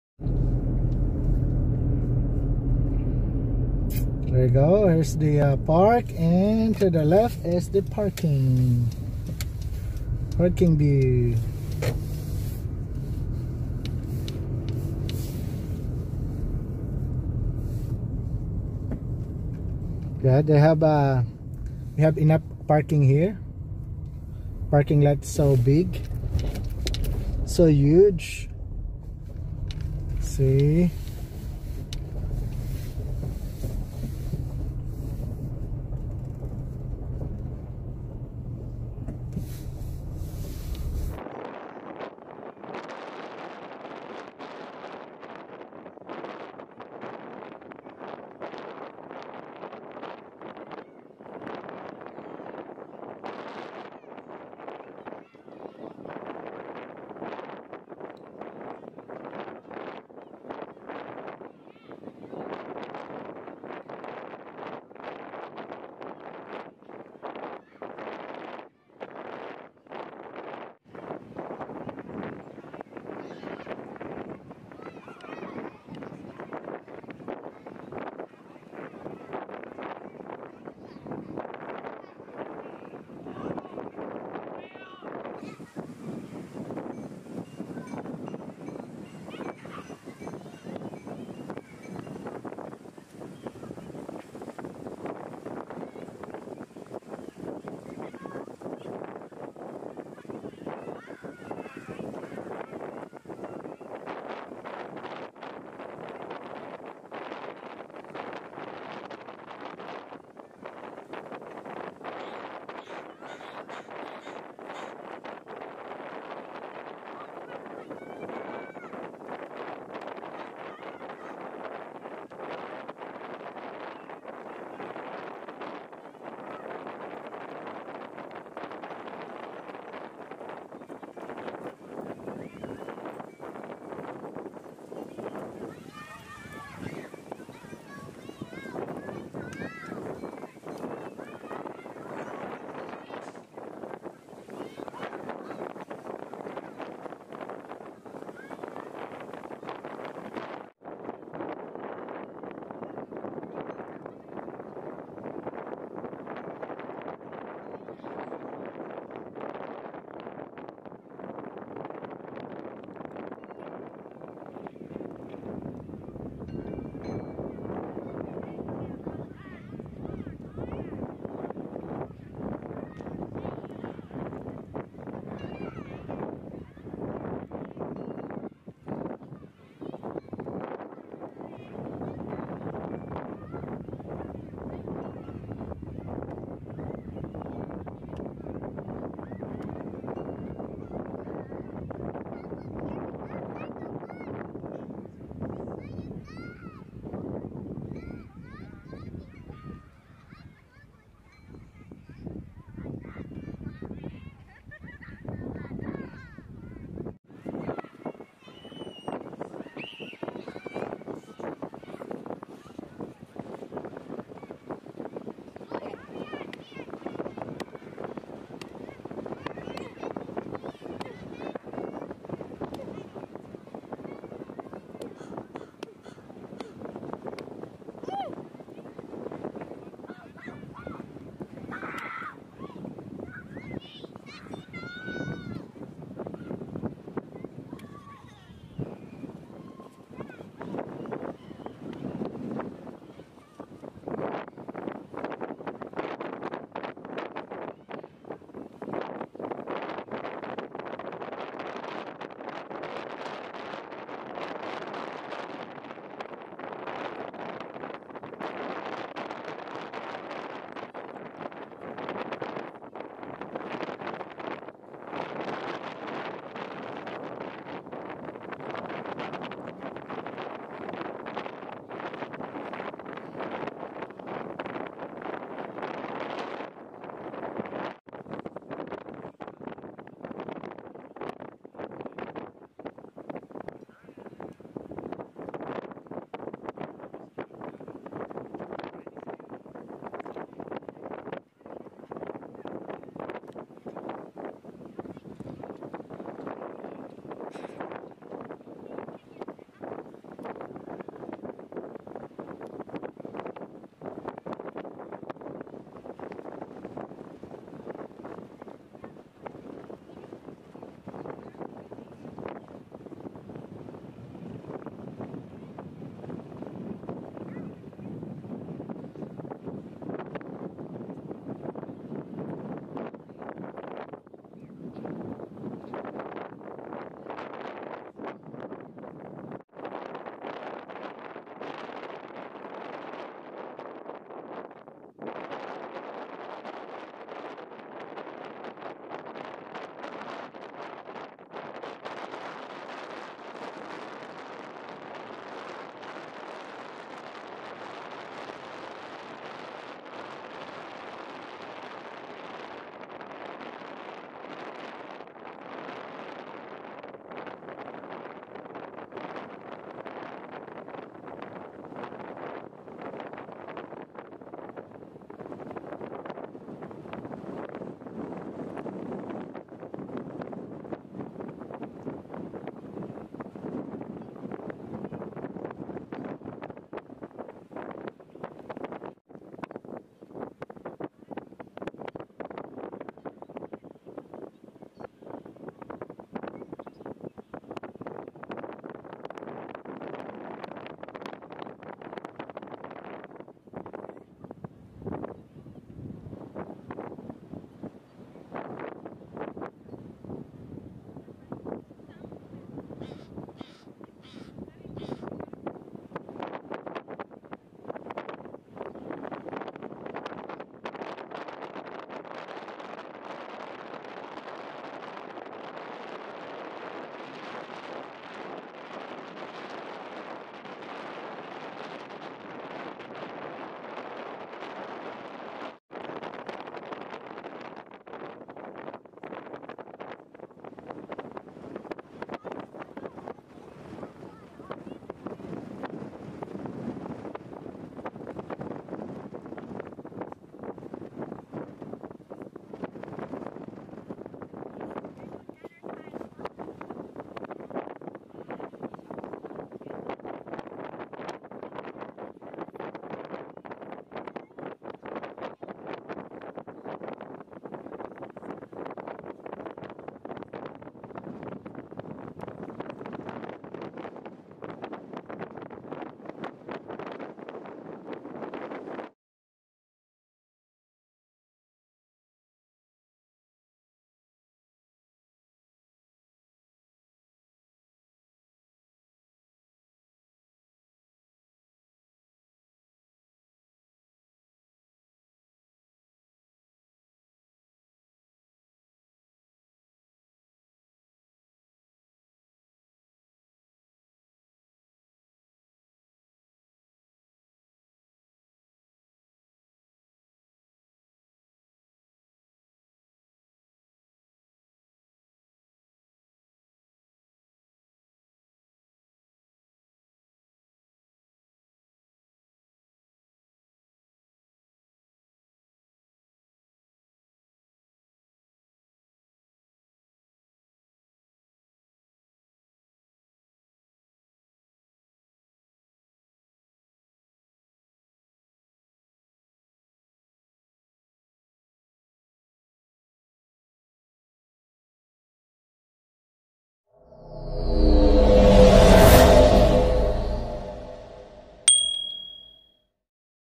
29🇨🇦 A sunny and windy day in Centennial Beach
Centennial Beach is a great place to explore all year round. Enjoy their sandy Beach and playground.  Kids will absolutely enjoy there. Great for bird watchers too. Make sure to bring extra jacket as it is always windy there.